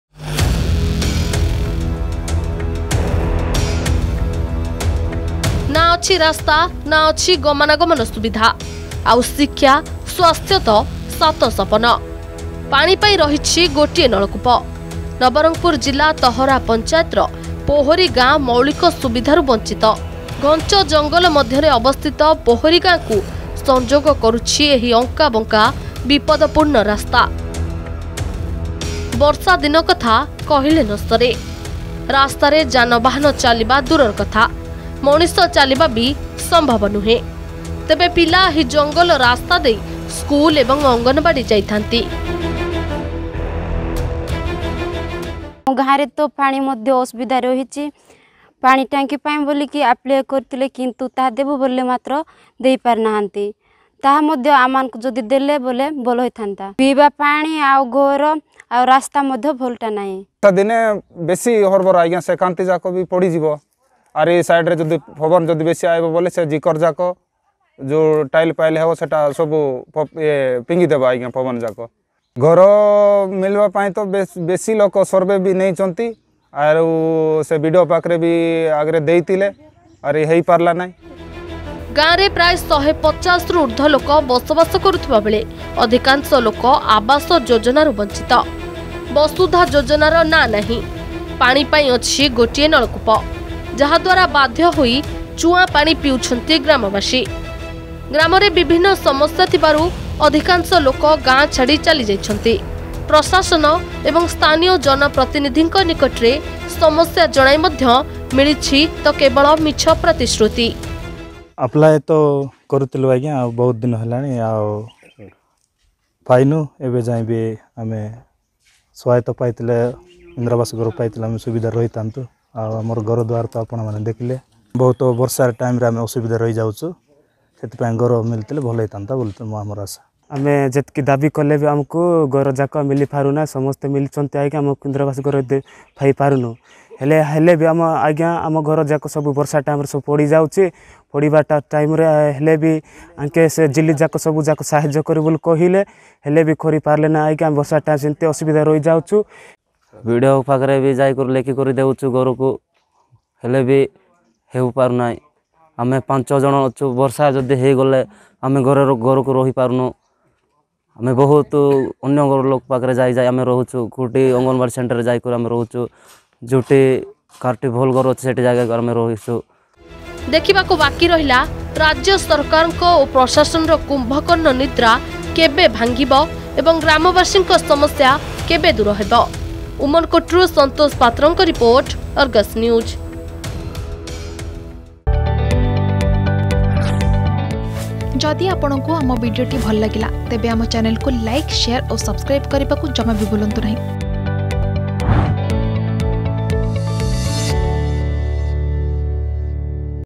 ରାଜ୍ୟ ସରକାରଙ୍କ ଗାଁ ଗାଁର ବିକାଶରେ ନାରା କେବଳ ପ୍ରହସନ । || Nabarangpur Govtproject scam
ରାଜ୍ୟ ସରକାରଙ୍କ ଗାଁ ଗାଁର ବିକାଶରେ ନାରା କେବଳ ପ୍ରହସନ । ନବରଙ୍ଗପୁର ଜିଲ୍ଲା ଉମରକୋଟ ବ୍ଲକ ତହରା ପଞ୍ଚାୟତର ପହରି ଗାଁରେ ନାହିଁ ମୌଳିକ ସୁବିଧା। ଶିକ୍ଷା, ସ୍ୱାସ୍ଥ୍ୟ, ଗମନାଗମନ, ବିଶୁଦ୍ଧ ପାନୀୟ ଜଳ ଯୋଗାଇବାରେ ରାଜ୍ୟ ସରକାର ଫେଲ୍ ମାରିଛନ୍ତି ।